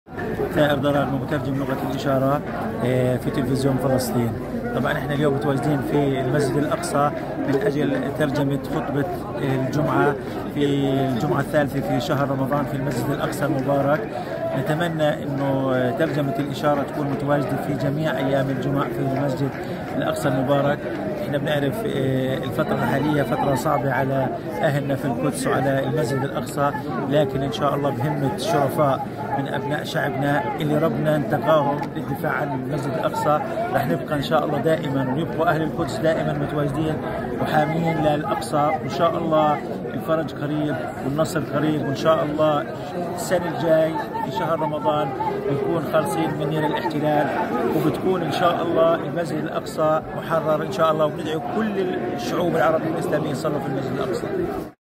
ثائر ضراغمة مترجم لغة الإشارة يتحدث عن ترجمة خطبة الجمعة الثالثة من رمضان في الأقصى
ثائر ضراغمة مترجم لغة الإشارة في تلفزيون فلسطين:" تواجدنا اليوم في الأقصى لترجمة خطبة الجمعة بلغة الإشارة ونتمنى أن تكون لغة الإشارة متواجدة في جميع أيام الجمع في الأقصى.. وان شاء الله رمضان الجاي بنكون خالصين من الاحتلال"